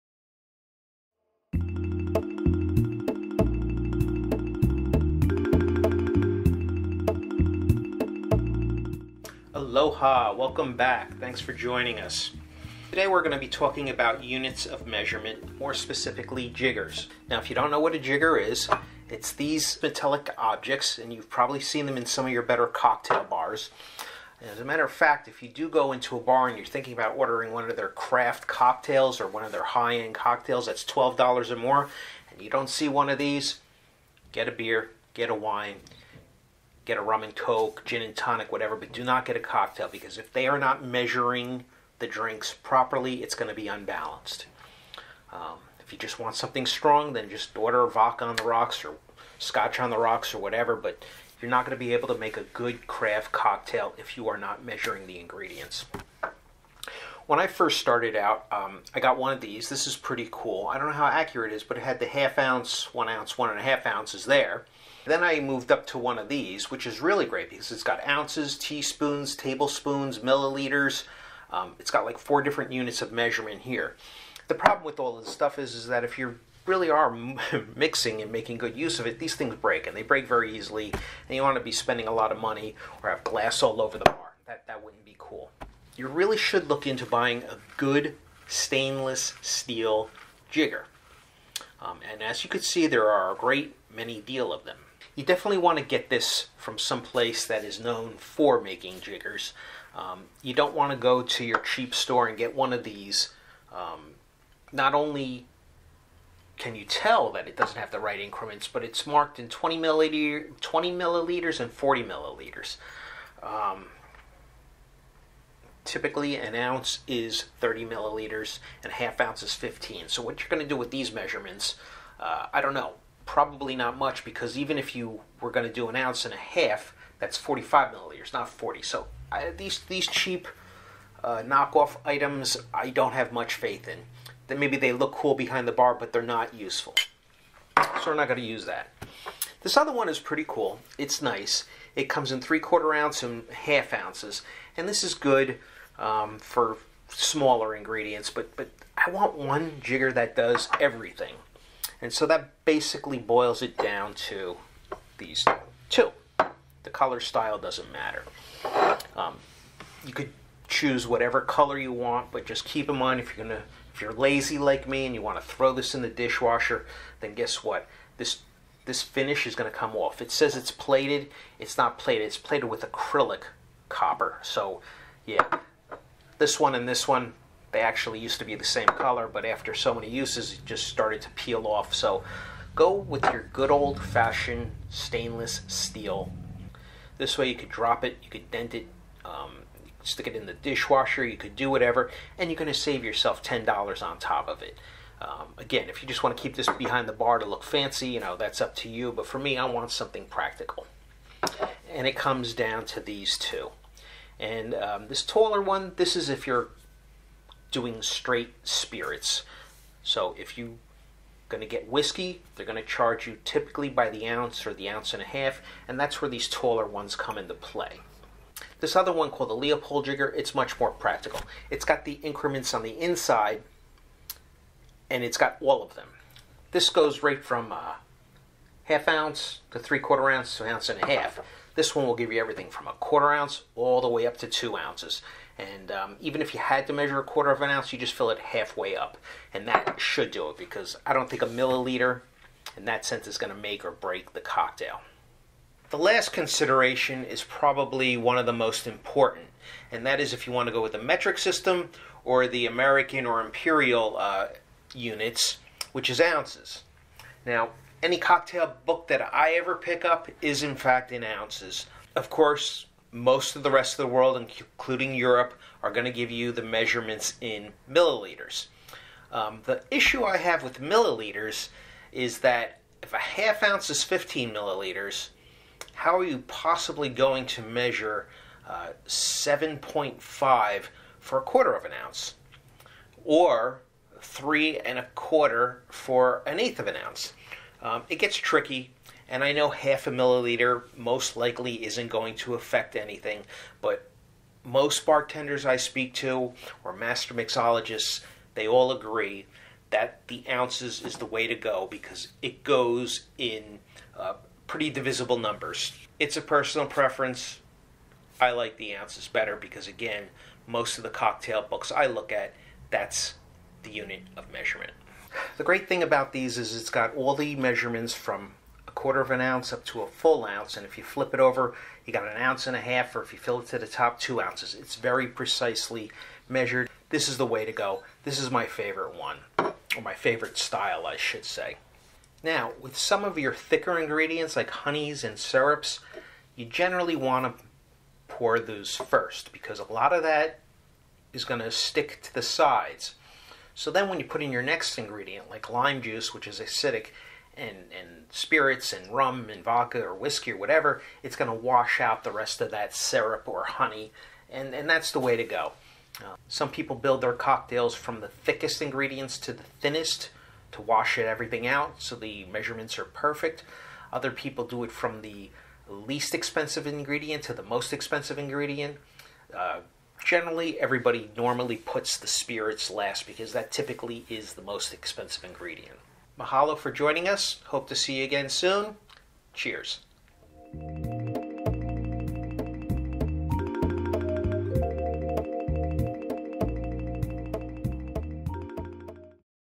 Tiki Triangle #2012 - Jiggers
Tiki Triangle Episode #2012 - Jiggers & Measurements; how to select a jigger and how to use it.